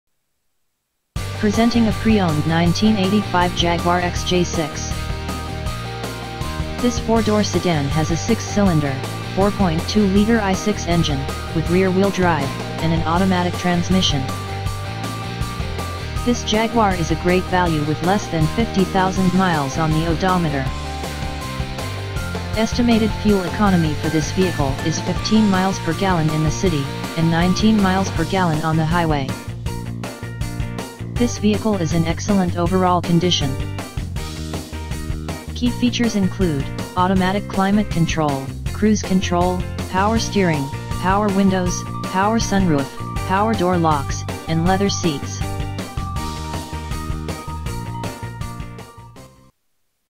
1985 Jaguar XJ6 XJ6 for sale in Delray Beach, FL
THIS ONE OWNER 1985 JAGUAR XJ6 IS A BEAUTIFUL EXAMPLE OF A RARE FULLY LOADED CLASSIC CAR !  ALL ORIGINAL, IN VERY GOOD CONDITION. ONLY 49K MILES THE EXTERIOR IS A LOVELY DARK GREEN WITH THE LEATHER INTERIOR A NICE TAN WITH WOOD TRIM! AND SUN ROOF. THIS JAGUAR DRIVES VERY SMOOTH, AND WITH MUCH STYLE. TRADES ACCEPTED.